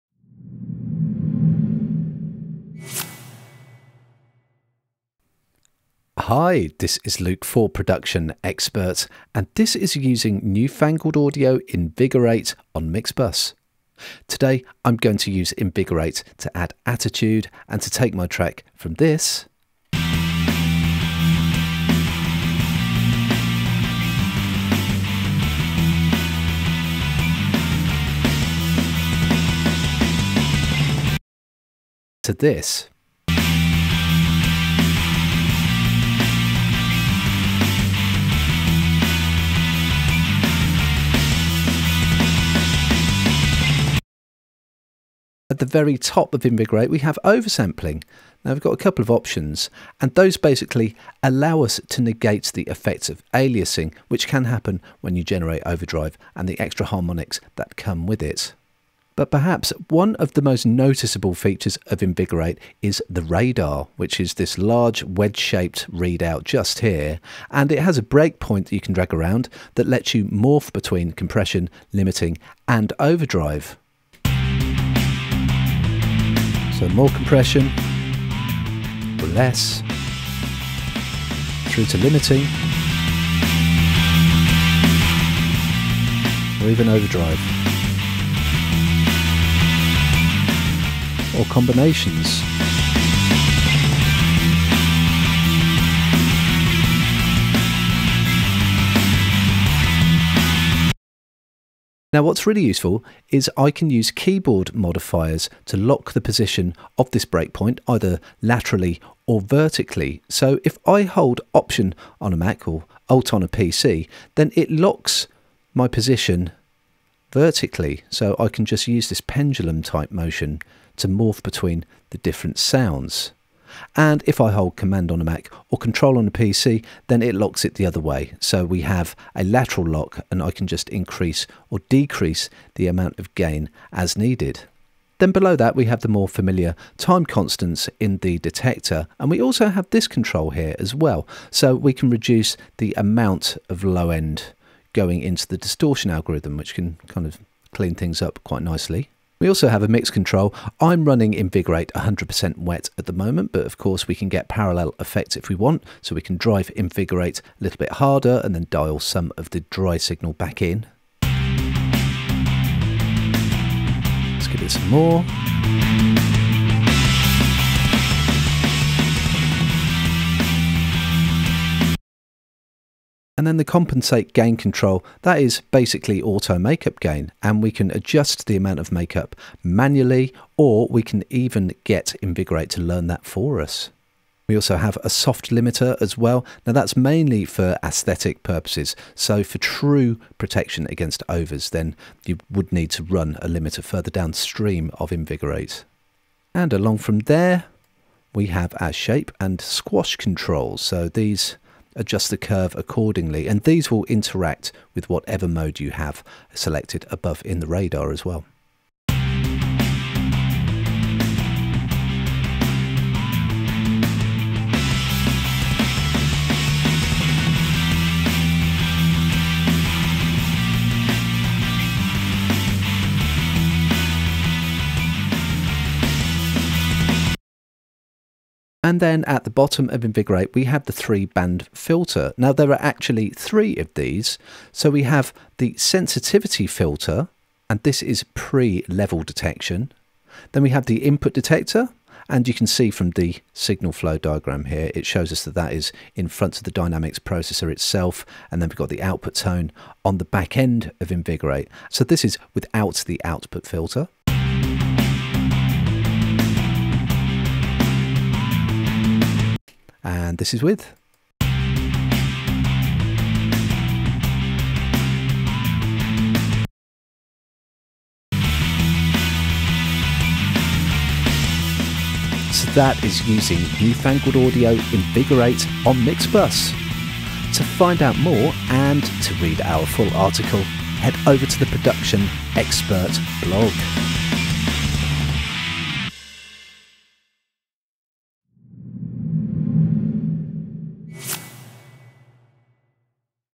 Using Newfangled Audio Invigorate On Mix Buss
What mix buss processing should entail can mean different things to different engineers. We take a listen to one that not only offers three processors, but also offers command over all of them of them with just one control…

Head over to the Production Expert blog to find out more…